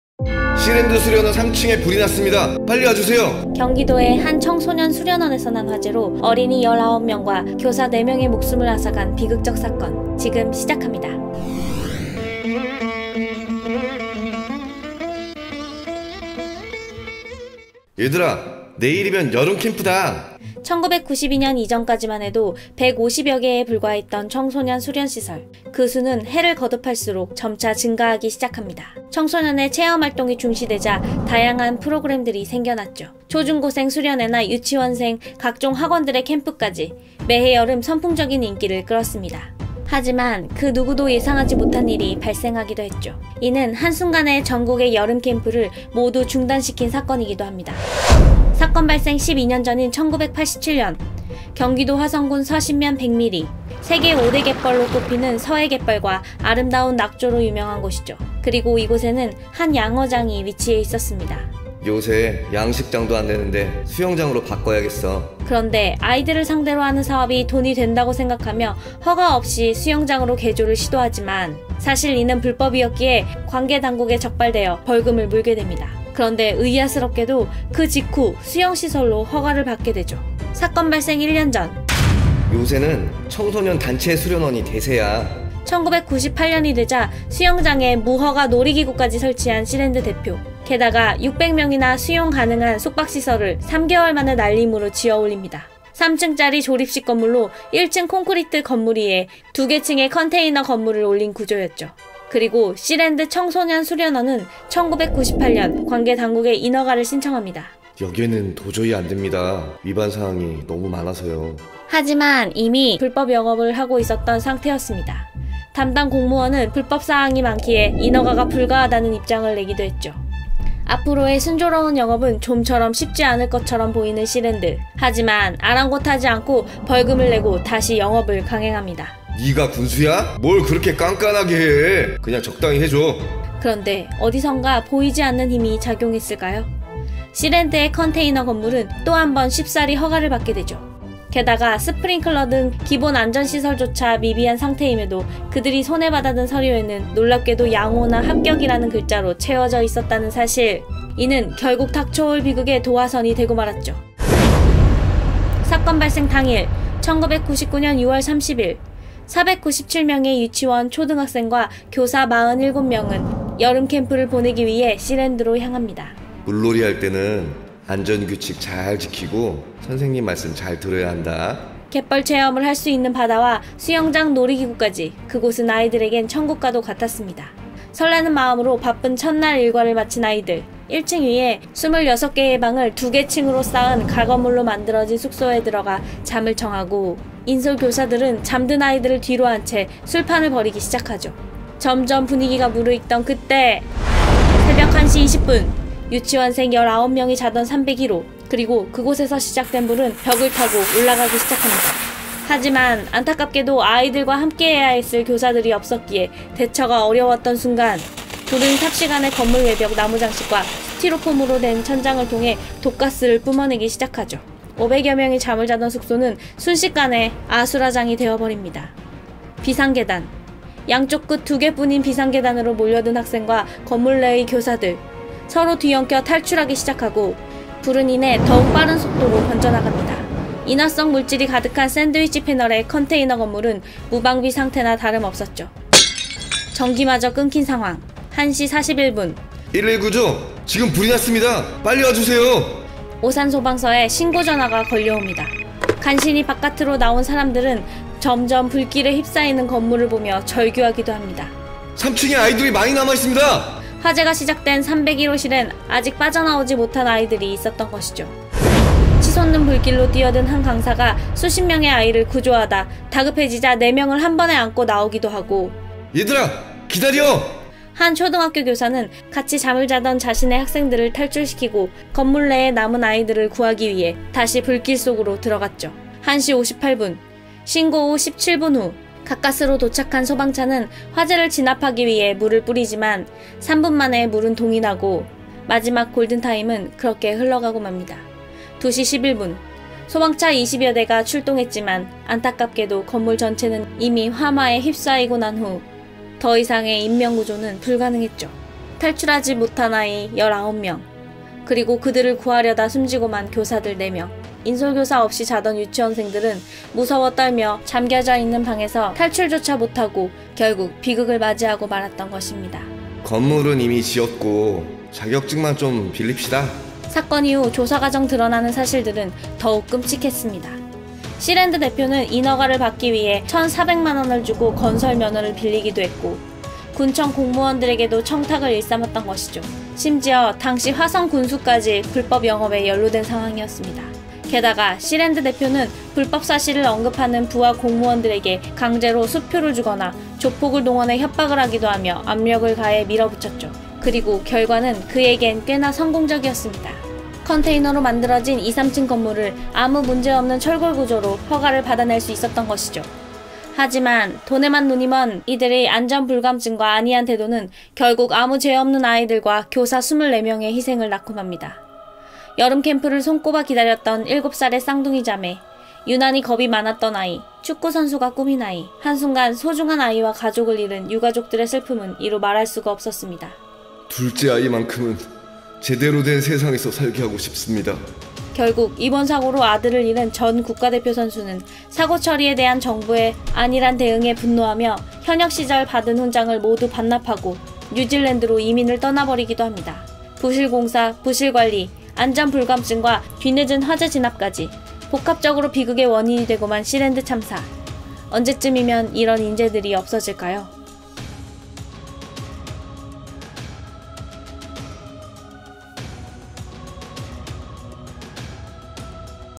씨랜드 수련원 화재 참사. 사라진 아이들의 꿈.
경기도의 한 청소년 수련원에서 난 화재로 어린이 19명과 교사4명의 목숨을 앗아간 비극적  사건, 지금 시작합니다.